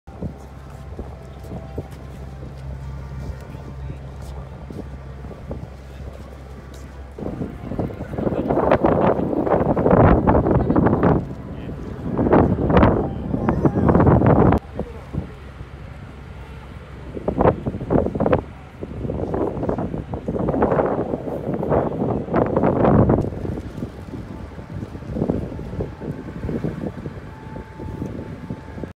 海上自衛隊救難飛行艇 US-2 三沢基地航空祭2022
日本が世界に誇る救難飛行艇、US-2です。実際に間近で見ると、その大きさに圧倒されます。両翼の下につけられた大きなフロート、機体下部ではなく横腹に収納される車輪、他の飛行機と異なる点が多くあります。アメリカ軍も同種の飛行艇の購入を検討しているとか。